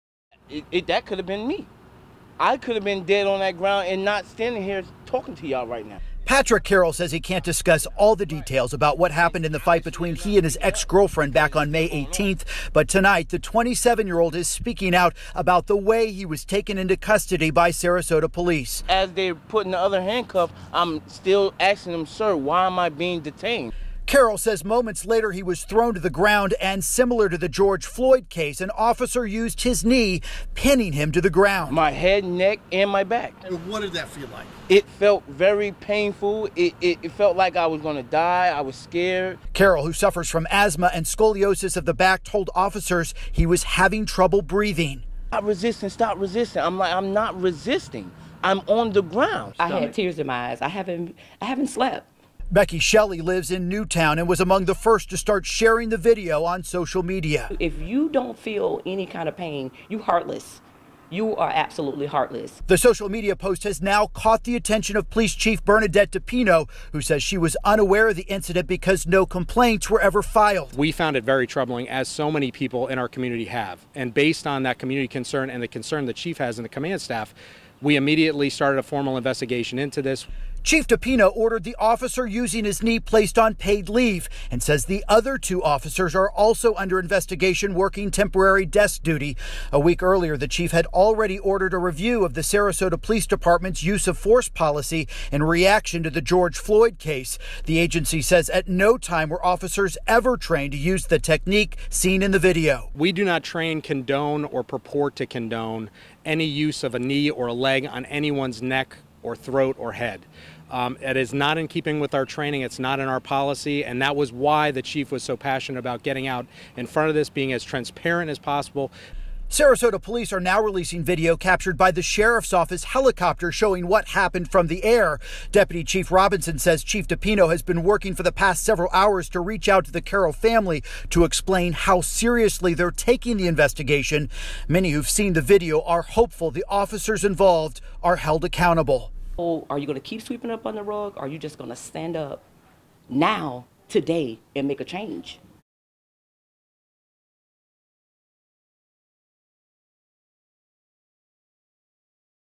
Sarasota police release video after officer accused of kneeling on man's neck
The Sarasota Police Department is investigating after one of its officers was accused of kneeling on the head and neck of a man being arrested.